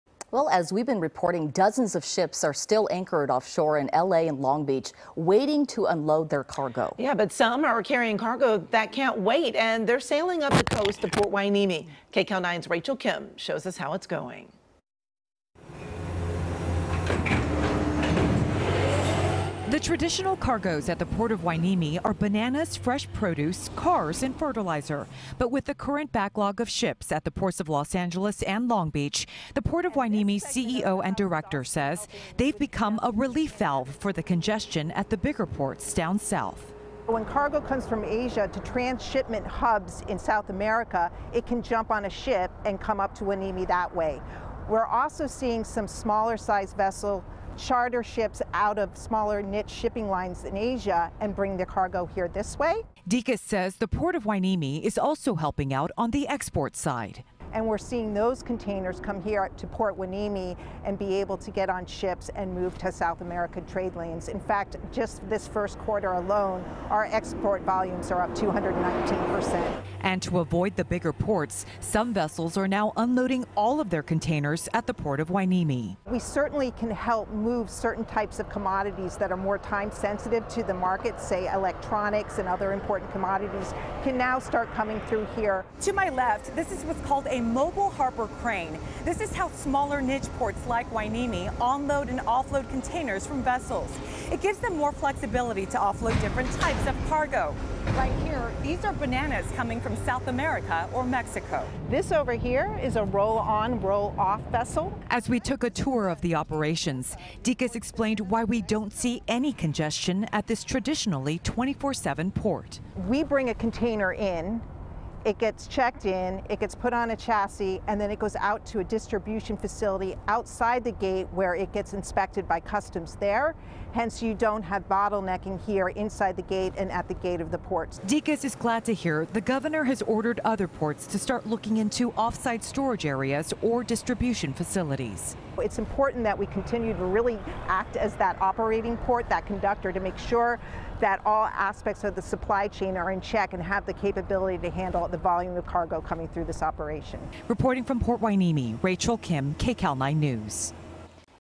Port Hueneme Acting As 'Relief Valve' For Crowded Ports Of LA And Long Beach
Rachel Kim reports from Port Hueneme where some cargo ships are going to avoid particularly long waits at the larger Ports of LA and Long Beach.